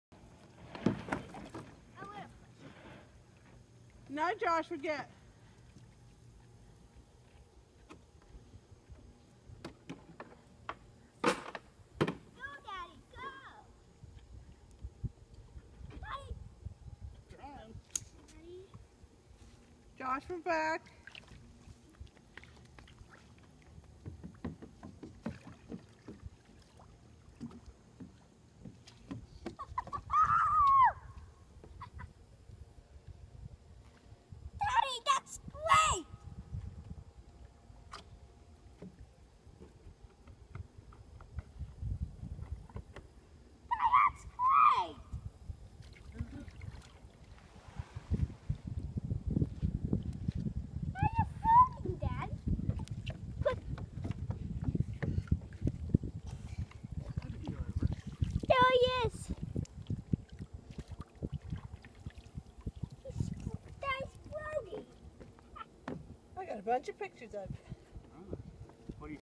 Fat guy in a little Boat
Got my new ocean kayak-trident 13 & trying it out for the first time